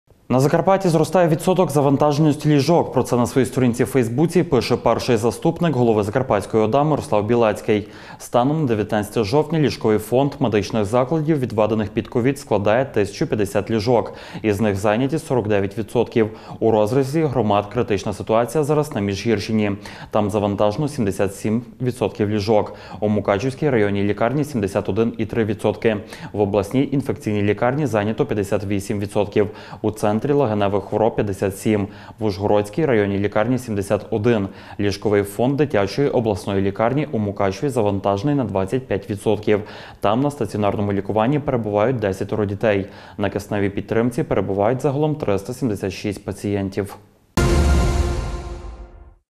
Зростає відсоток завантаженості ліжок.Covid-ситуація
На Закарпатті зростає відсоток завантаженості ліжок. Про це на своїй сторінці у фейсбуці пише перший заступник голови Закарпатської ОДА – Мирослав Білецький.Станом на 19 жовтня ліжковий фонд медичних закладів, відведених під ковід, складає 1050 ліжок, з них зайняті - 49%.У розрізі громад критична ситуація зараз на Міжгірщині, тут завантажено 77% ліжок, у Мукачівській районній лікарні – 71,3%.В Обласній інфекційній лікарні зайнято 58% ліжок, у Центрі легеневих хвороб – 57%, в Ужгородській районній лікарні – 71%.Ліжковий фонд Дитячої обласної лікарні у Мукачеві завантажений на 25%, тут на стаціонарному лікуванні перебувають 10 дітей. На кисневій підтримці перебувають 376 пацієнтів.